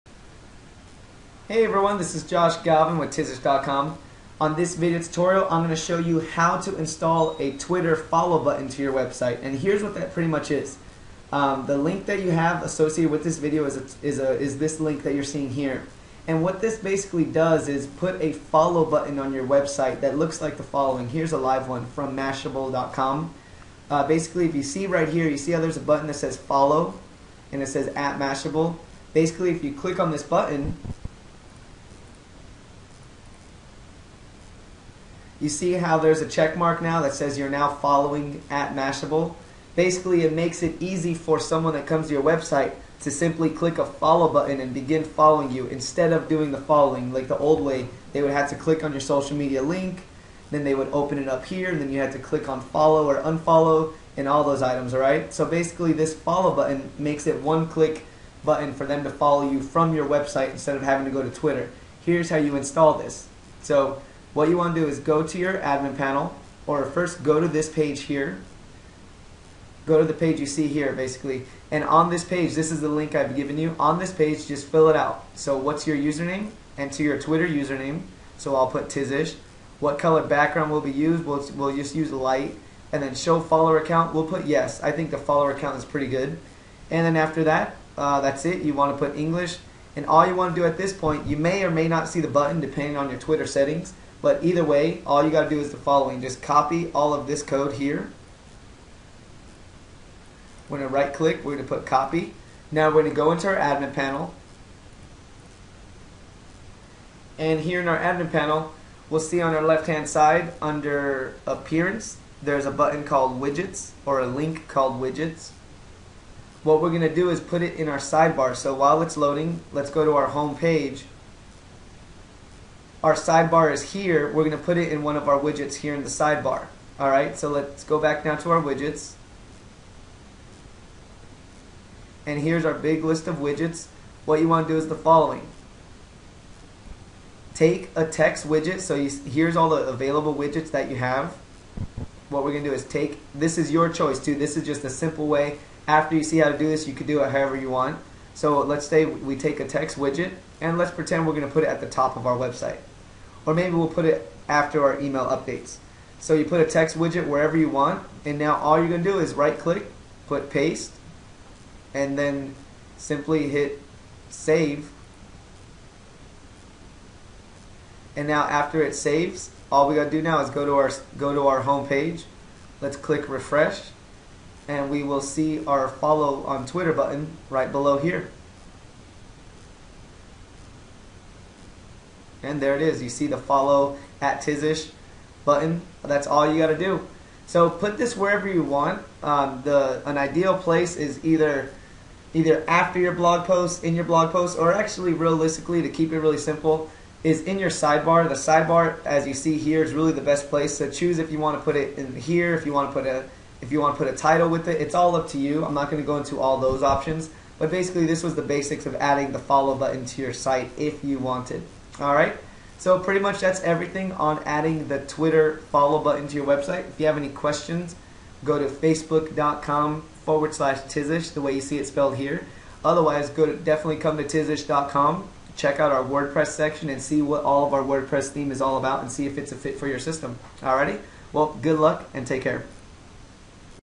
How To Install The Twitter Follow Button On Your Wordpress Blog
- In this video tutorial you will learn how to put a Twitter follow button that allows visitors to your website to instantly follow you on Twitter without having to go to your Twitter page to do so.